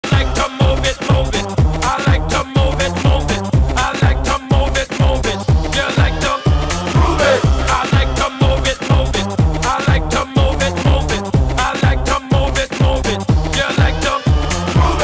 Parrots Dancing and Flying! BirdNerdSophie
I hope you like this short of my cockatiels and conures dancing, flying and living their best lives!

All profits earned through sales will go directly to the birds for their care.